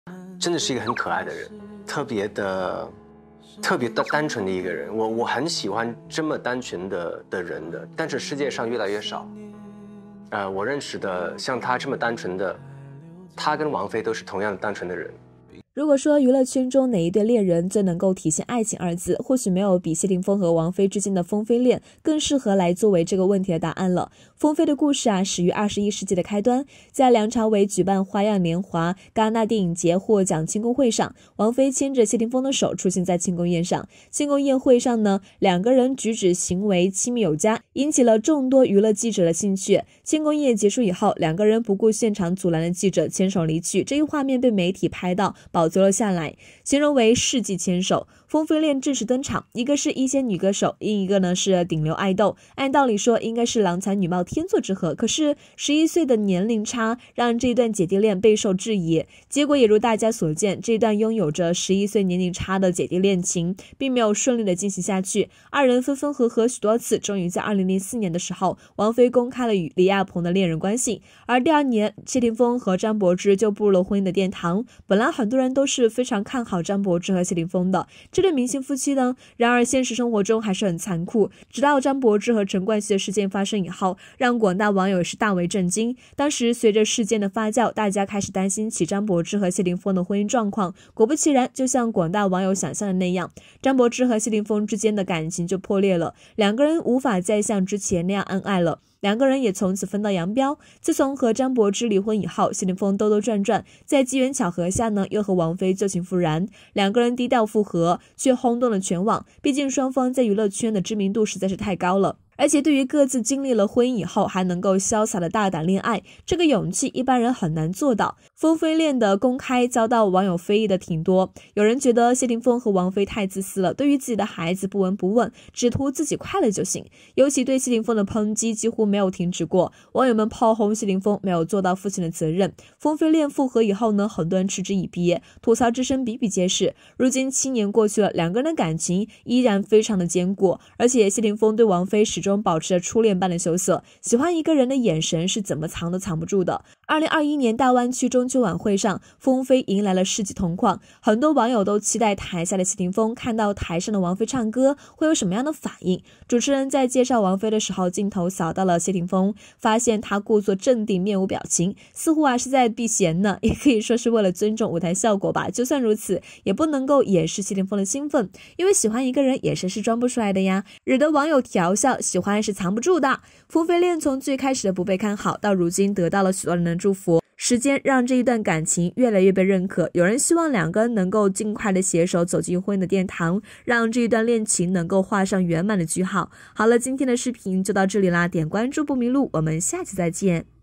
王菲后台偶遇谢霆锋，请注意她下意识的反应，喜欢是藏不住的
如果说，娱乐圈中的哪一对恋人，最能够体现“爱情”二字，或许没有比谢霆锋和王菲之间的“锋菲恋”，更适合来作为这个问题的答案。
锋菲的故事始于二十一世纪的开端，在梁朝伟举办《花样年华》戛纳电影节获奖庆功会。王菲牵着风的手出现在庆功宴上 ，庆功宴会上两人举止行为亲密有加，引起了一众多娱乐记者的兴趣，庆功宴结束后，两人不顾现场阻拦的记者，牵手离去，这一画面被媒体拍照保留了下来，形容为“世纪牵手”，“锋菲恋”正式登场。一个是一线女歌手 ，而另一个是顶流爱豆，按道理说应该是郎才女貌天作之和，可是11岁的年龄差，让这段姐弟恋备受质疑。
结果也如大家所见，这段拥有着11岁年龄差的姐弟恋情，并没有顺利的进行下去，二人分分合合许多次，终于在2004年的时候 ，王菲公开与李亚鹏的恋人关系，而第二年谢霆锋与张柏芝就步入了婚姻的殿堂